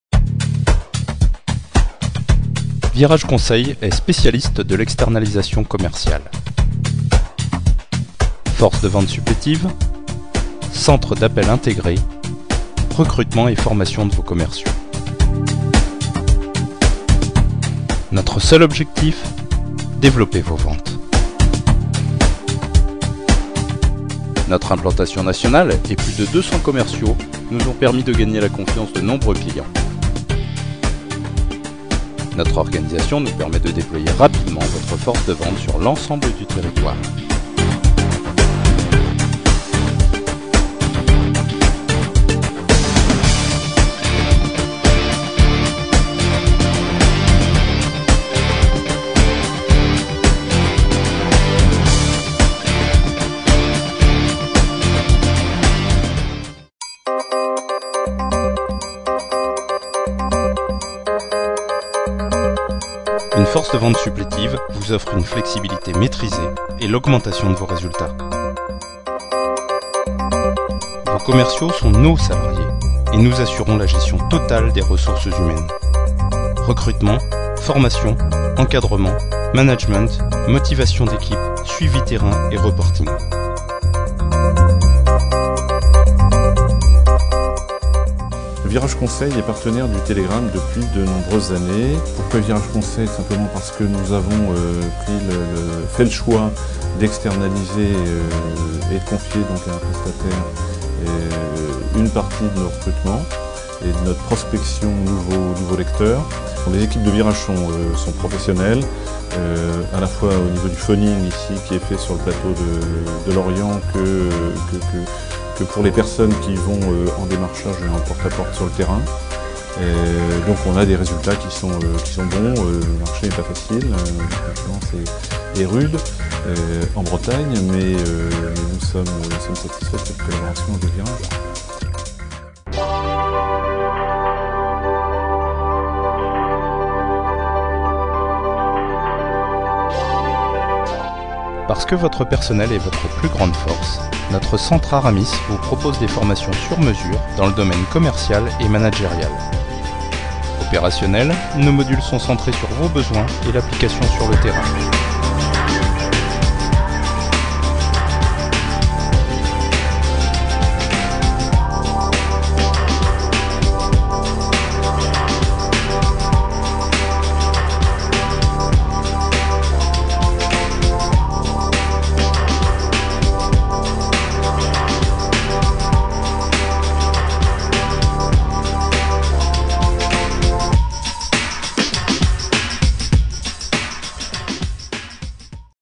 Virage Conseil
Le film de Virage Conseil, est en ligne sur son nouveau site internet. Ce spécialiste de la Force de Vente depuis 1993, basé à Lorient (Morbihan 56) mais développant son expertise sur tout le territoire national, a confié à e-mage concept la réalisation de ce clip qui met présente de manière dynamique les activités et les nombreux atouts de cette société